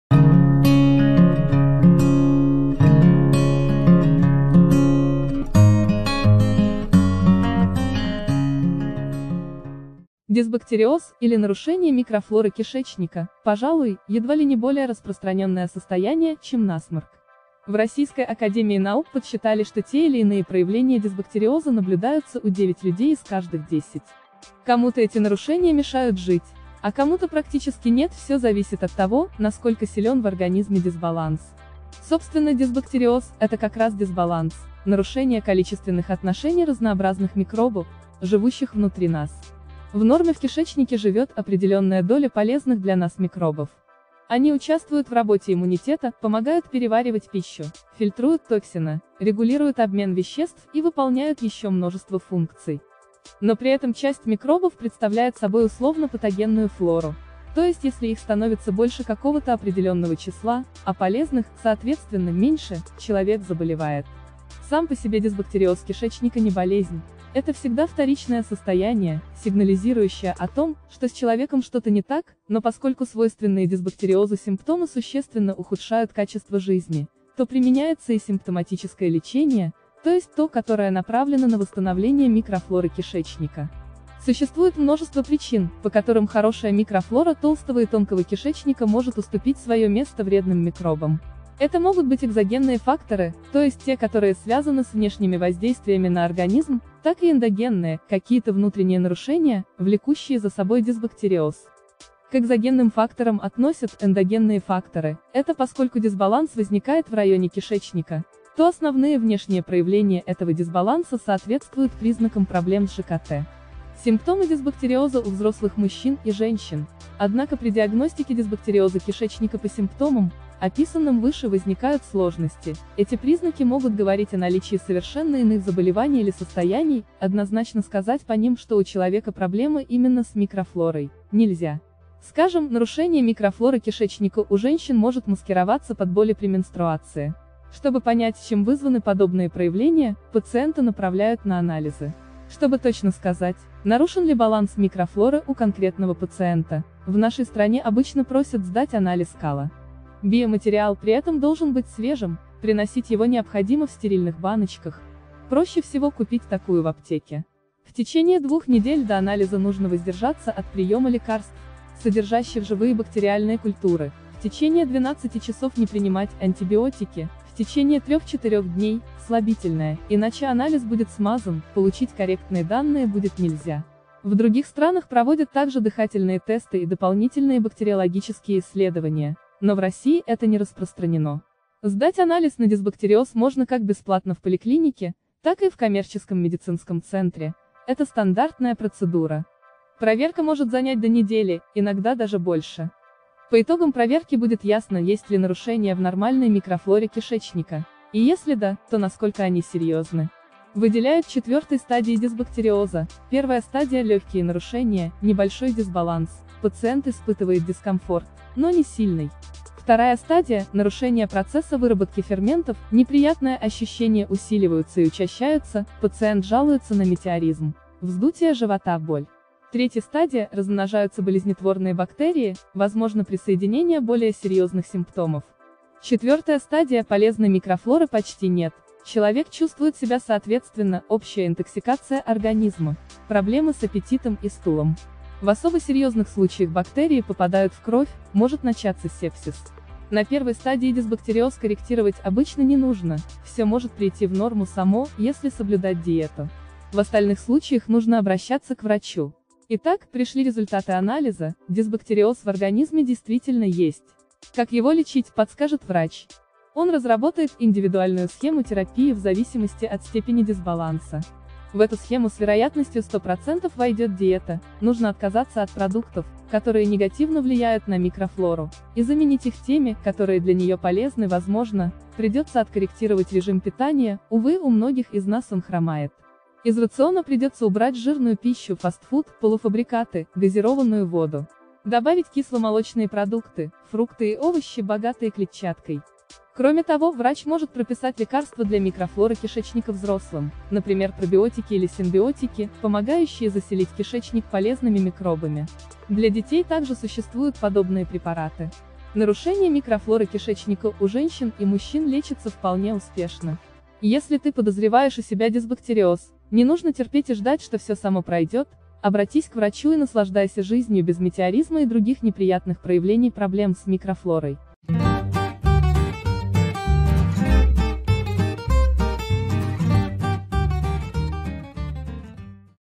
Дисбактериоз кишечника что это такое чем вредно как проявляется и как лечить
Дисбактериоз, или нарушение микрофлоры кишечника, пожалуй, едва ли не более распространенное состояние, чем насморк.